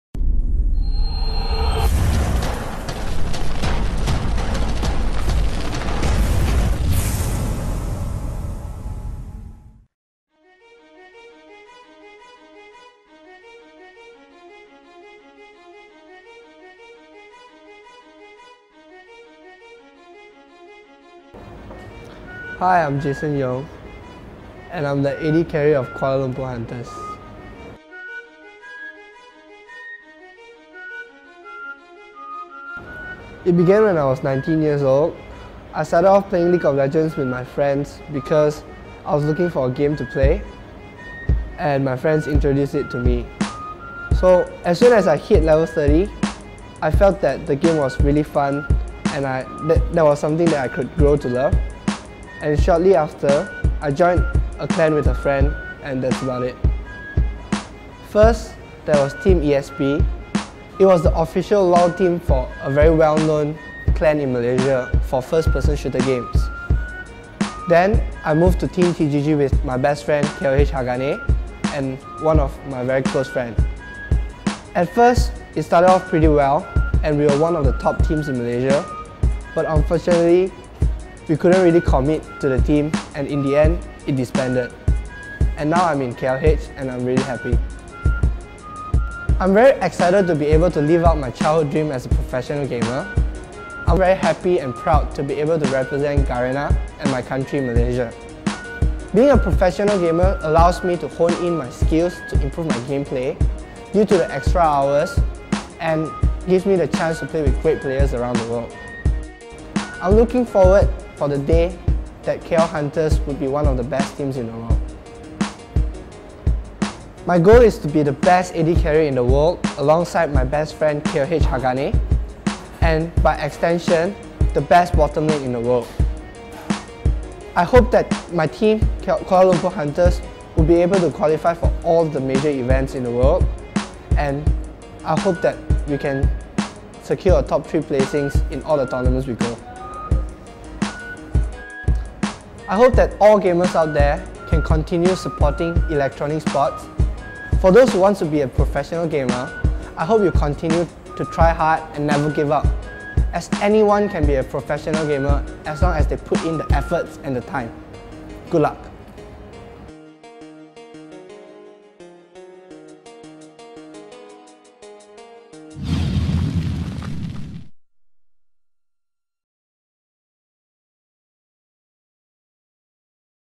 Celebrity Talks : JaeYoong on League of Legends
On this episode of celebrity talk, we get the chance to hear from our very own up and rising celebrity gamer, JaeYoong!

Find out about his history before joining the KL Hunters as well as his goals and inspiration as an E-Sports player.